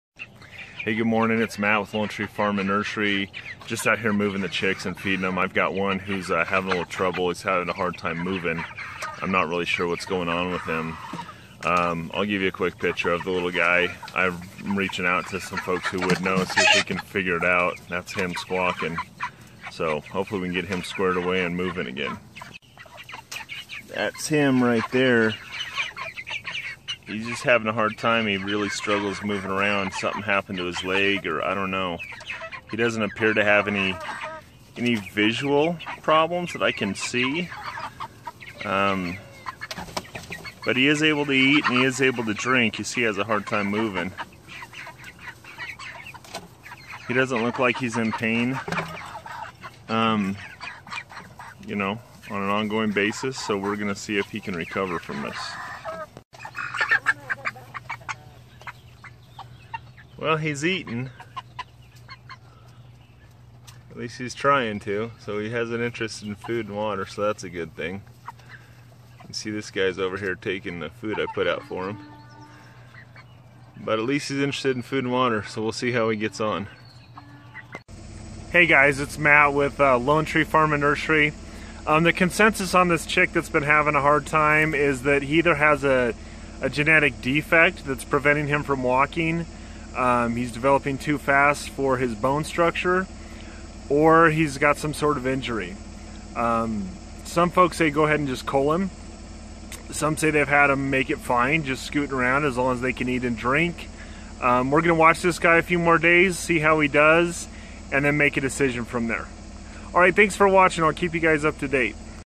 Vlog Day 9 - Broiler can't walk.
One of are broilers can't walk. He scoots around the pen. We're monitoring and trying to figure out what to do next. Could be a genetic leg defect or injury or he's going too fast for his bone structure. Some folks say they've raised them to butcher weight like this and other say to cull him right away. We're going to observe a little longer and then make a decision.